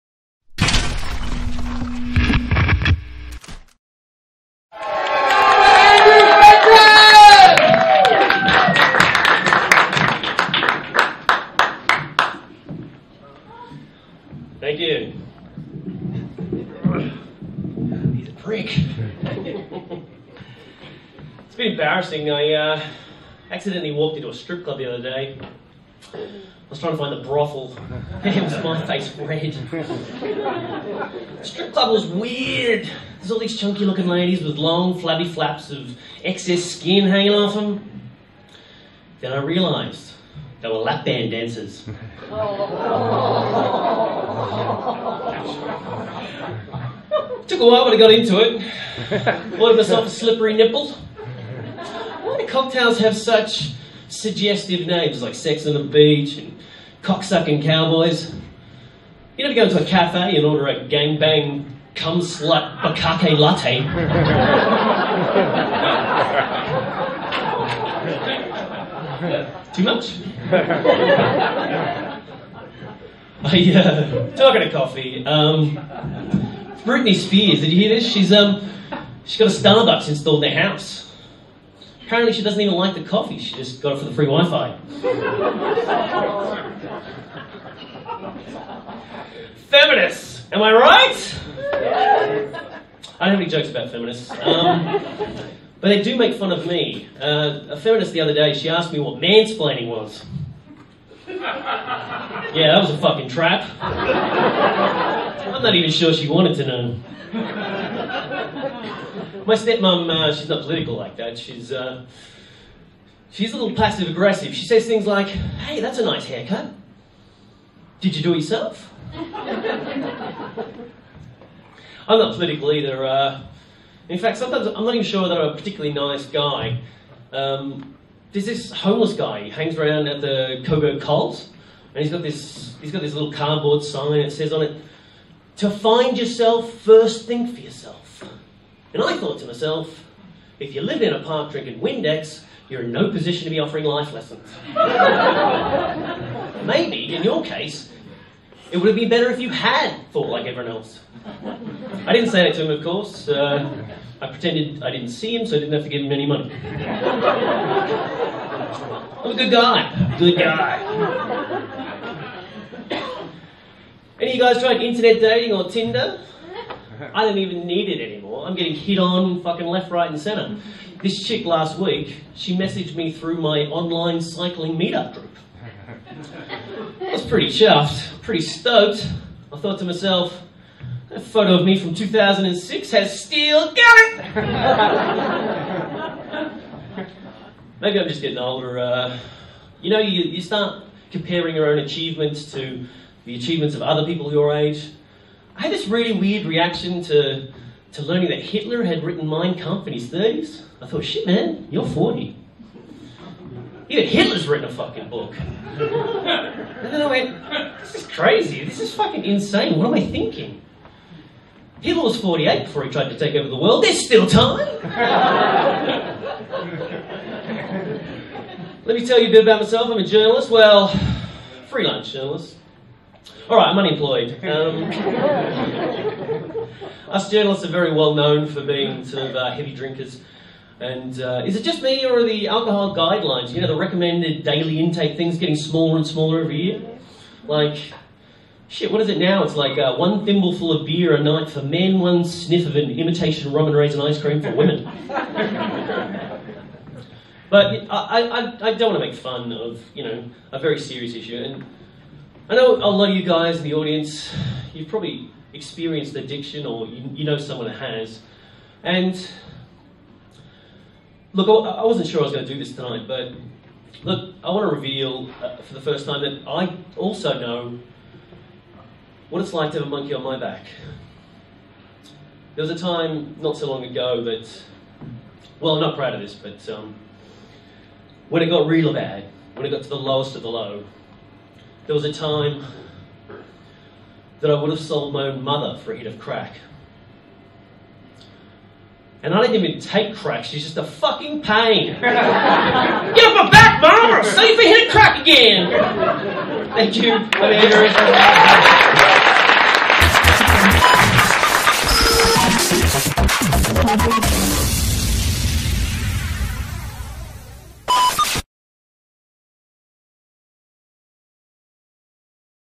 Andrew Fenton - School of Hard Knock Knocks - Learn Stand-up Comedy in Melbourne
Watch this incredible debut stand-up comedy performance by Andrew Fenton, at the School of Hard Knock Knocks graduation in July.